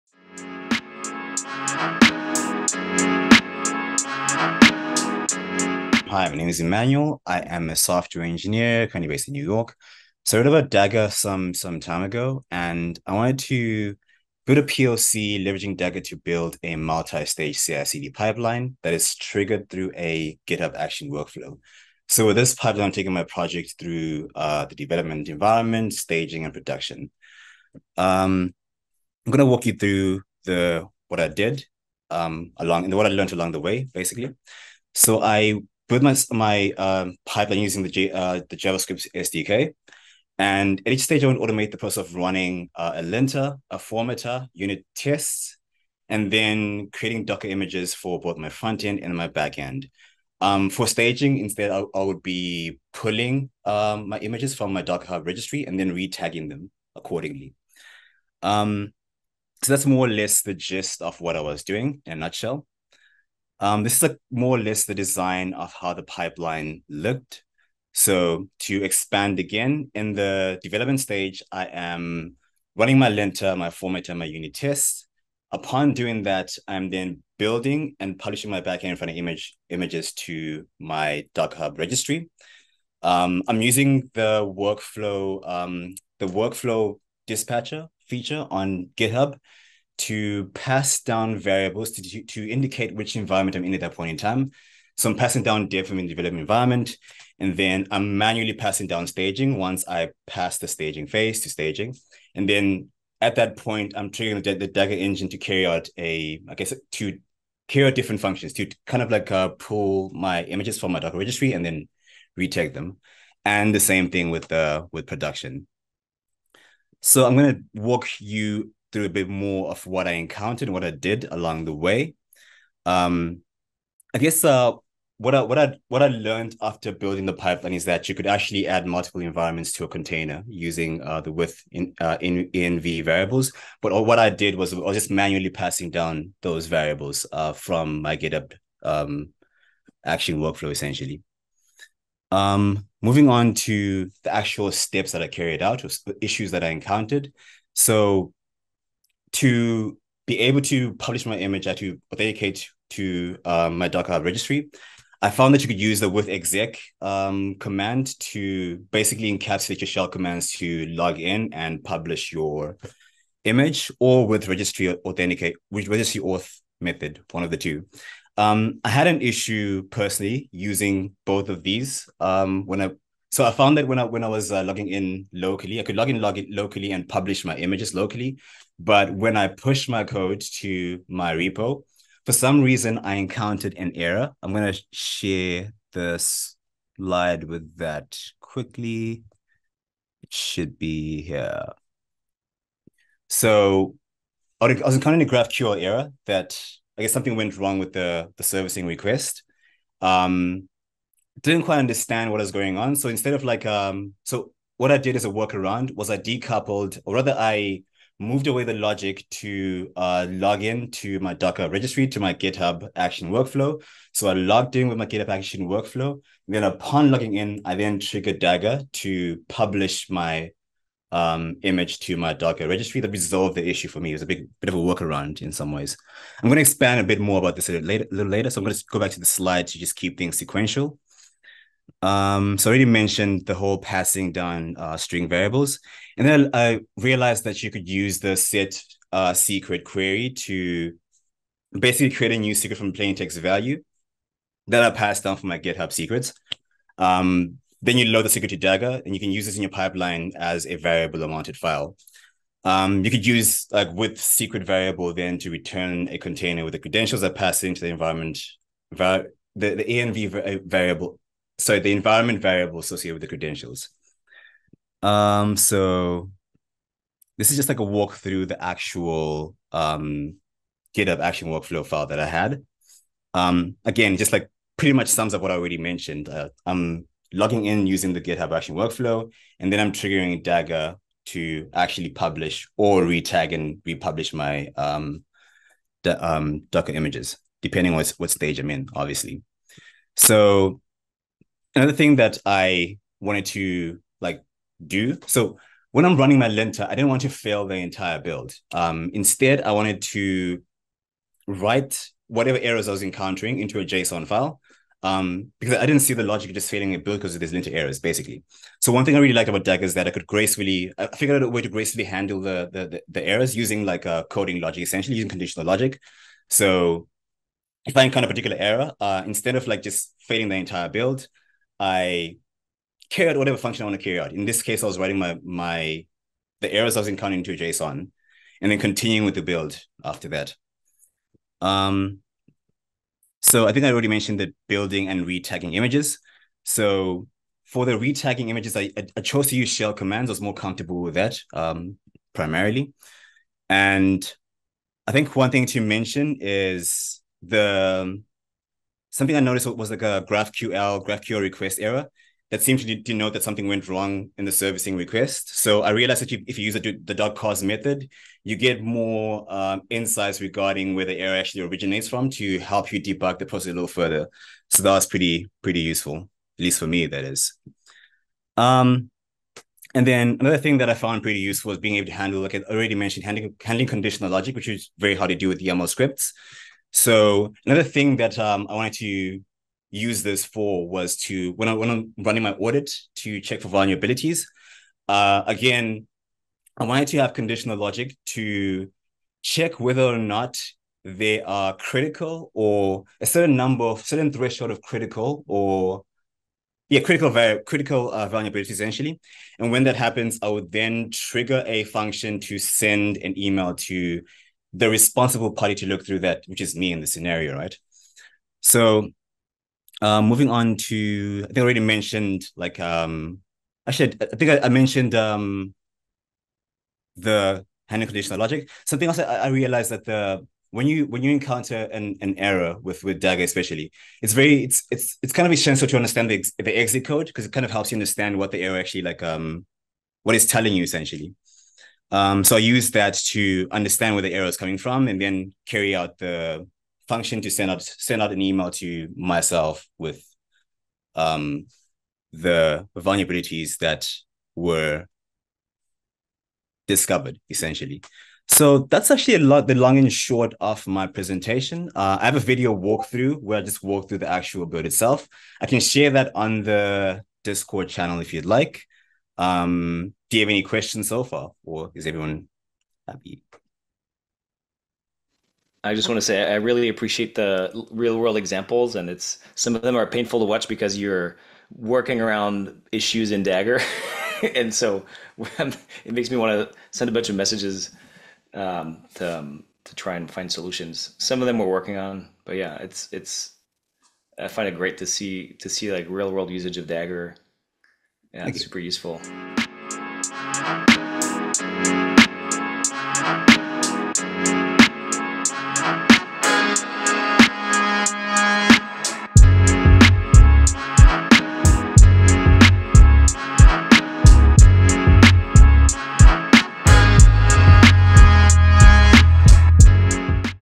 Leveraging Dagger to Build Multi-stage CI/CD Pipeline and Trigger through a GitHub Action Workflow
Emmanuel Sibanda, a software engineer, dives deep into his journey of building a multi-stage CI/CD pipeline triggered through a GitHub action workflow, leveraging the power of Dagger. He uses Dagger for his project to facilitate the automation of tasks such as linting, formatting, and unit testing using Python and JavaScript SDKs. Watch the demo to learn more about how Emmanuel got started with this POC, and where he is hoping to take it next!